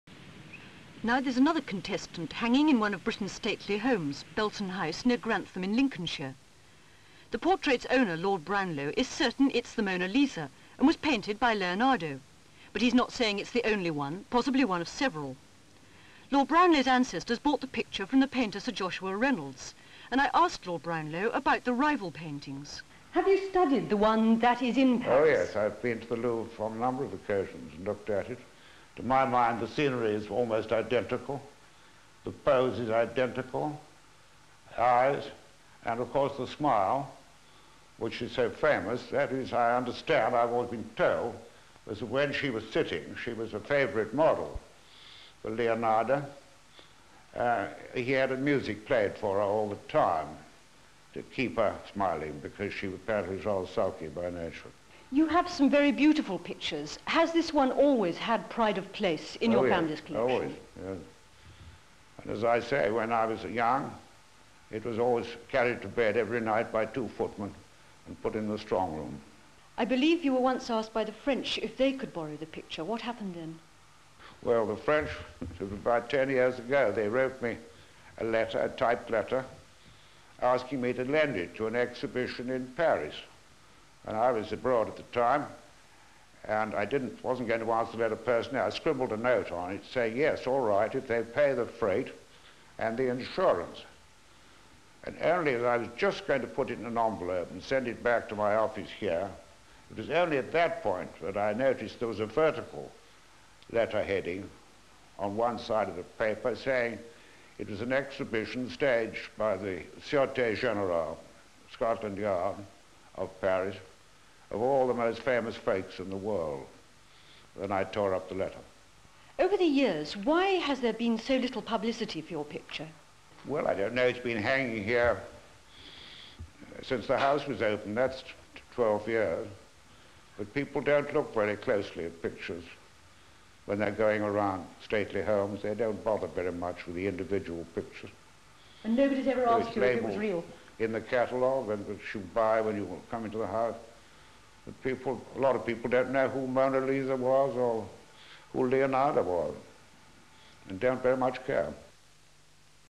SYND 16-10-72 INTERVIEW ON 'MONA LISA' PAINTING
(16 Oct 1972) An interview with Lord Brownlow - owner of yet another possible "original" Mona Lisa.